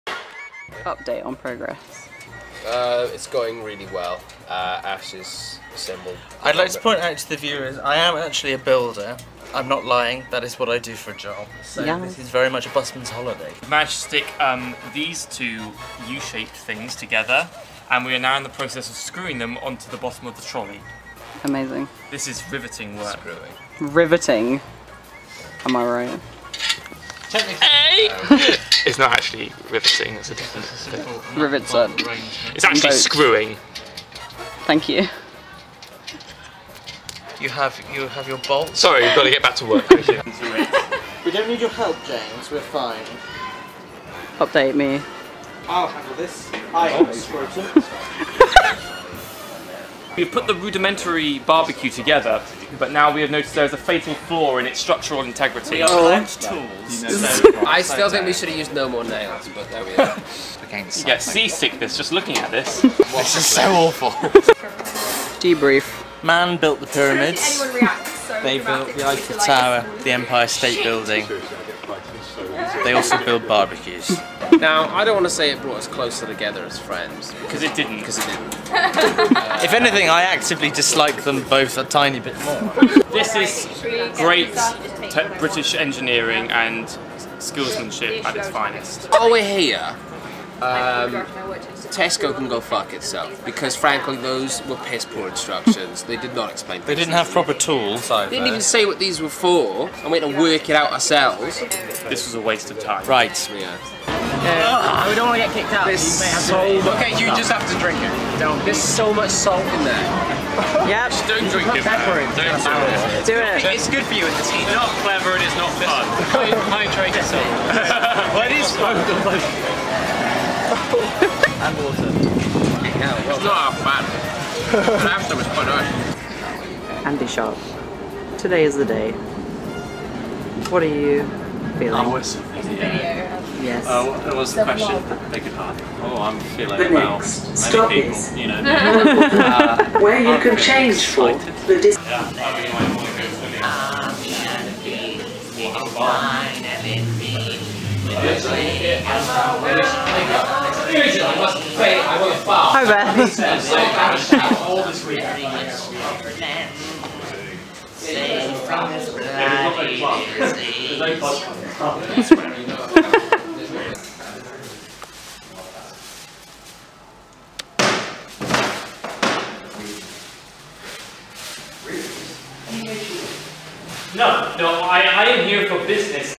The House of Edgar 2018 - PREVIEW WEEK
We have begun our run of the House of Edgar at the Edinburgh Fringe Festival, but here's a throwback to our previews in the New Wimbledon Theatre!

There is no actual theatre involved in this vlog.

Only shenanigans.